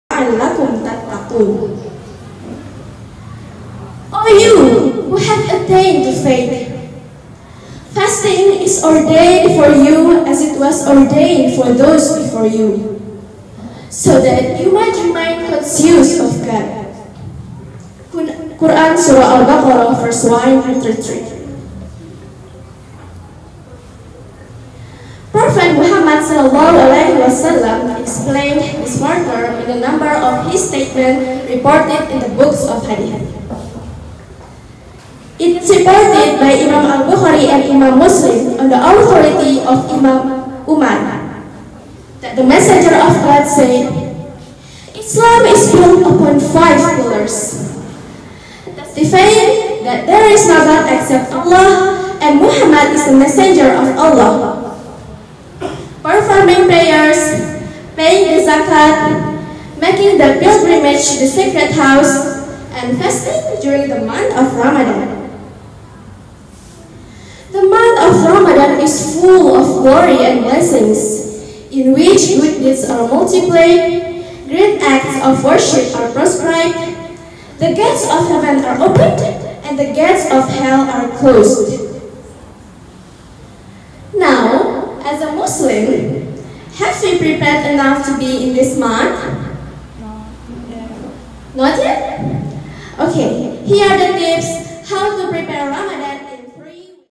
Spentega Contest 1st | Lomba Pidato
Pada video kali ini dilaksanakan kontes pidato dalam bahasa Inggris